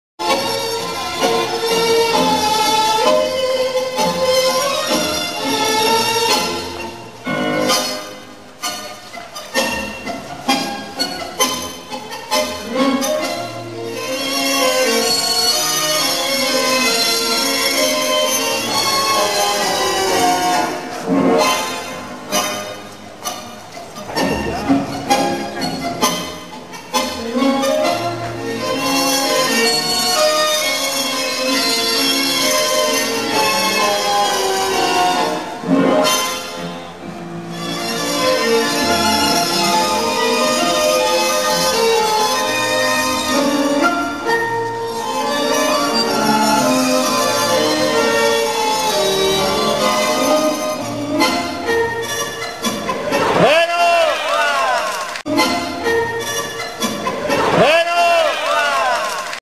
Argentine Tango Dance
Chicho stepping Biagi. Rodolfo Biagi was the pianist in D'Arienzo's orchestra in 1935, he created the immensely influential "electric" stacatto rhythm. Biagi refined and purified his music style even sharper in his own orchestra, it's a very sharp stacatto that electrifies the dancers.